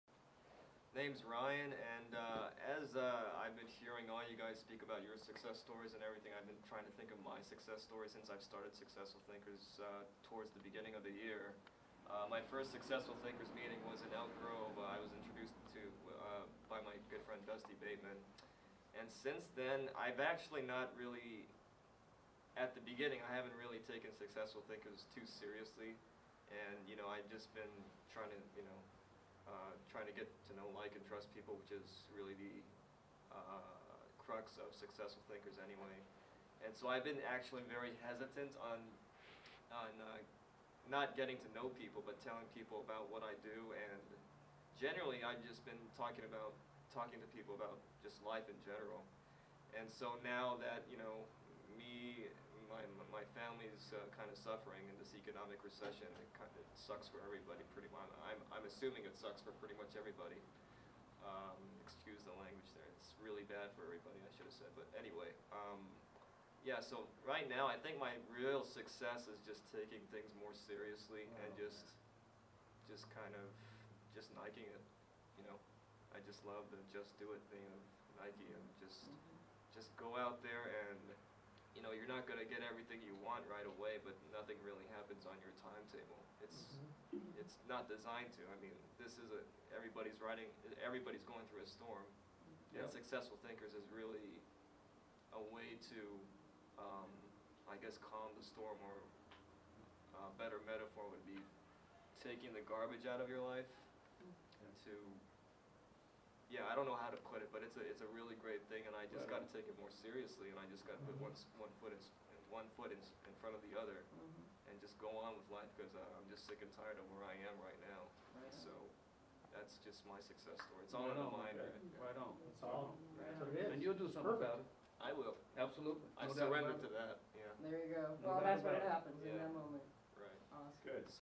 Ryan Success Story
shares a success story at Shingle Springs Successful Thinkers meeting on Oct 3, 2011